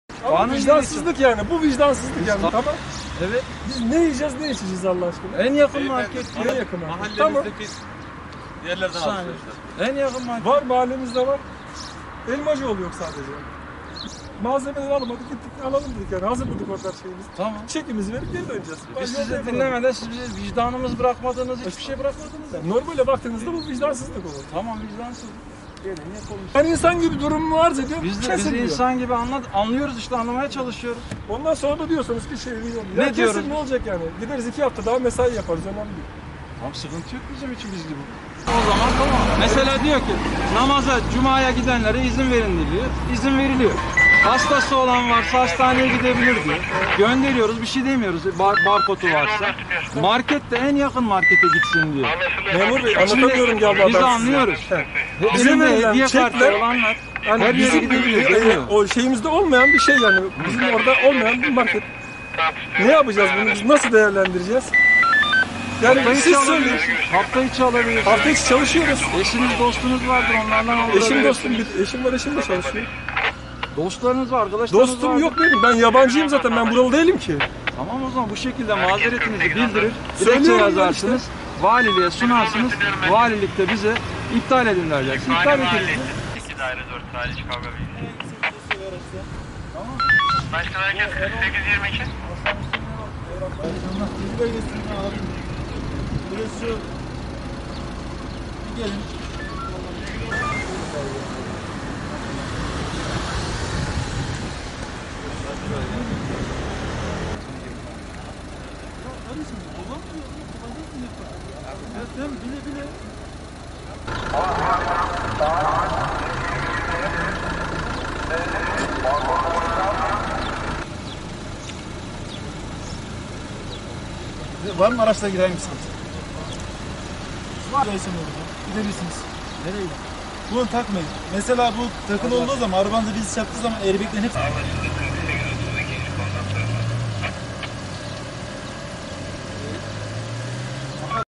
Kısıtlamayı deldi, ceza yedi, polise “Vicdansızlık” dedi
Aksaray’da sokağa çıkma kısıtlamasını delen ve aracıyla markete giderken polis ekiplerine yakalanan sürücü ceza yiyince görevli polis memurlarına “Bu vicdansızlık” diyerek tepki gösterdi.
Aksaray’da sokağa çıkma kısıtlamasında kent genelinde İl Emniyet Müdürlüğü ve İl Jandarma Komutanlığı tarafından geni güvenlik önlemleri alınırken şehirde adeta kuş uçurtulmadı. Çalışmalar kapsamında Taşpazar Mahallesi Ebulfeyz Elçibey Caddesinde uygulama yapan İl Emniyet Müdürlüğü Trafik Tescil ve Denetleme Şubesi ekipleri Cüneyt K. (49) idaresindeki 68 DY 874 plakalı Hyundai marka otomobili durdurdu. Sürücü ve izin belgesi sorgulaması yapan polis ekipleri sürücünün sokağa çıkma kısıtlamasını ihlal ettiğini tespit etti. Kısıtlamayı ihlal etmekten 3 bin 150 TL para cezası uygulanan sürücü görevi başındaki polis memurlarına “Vicdansızlık” diye tepki gösterdi. Polis memurları kısıtlama ve çalımalar hakkında bilgi vermek istese de sürücü ısrarla markete gitmek istediğini belirterek cezadan kurtulmaya çalıştı. “Biz ne yiyeceğiz? Ne içeceğiz?” diye polis memurlarına soran sürücü polis memurlarının “En yakın market” demesi üzerine, “Var, mahallemizde var. Ama şu market yok sadece” diye cevap verdi. Sürücüye kısıtlamayı ihlalden 3 bin 150 TL para cezası uygulandı.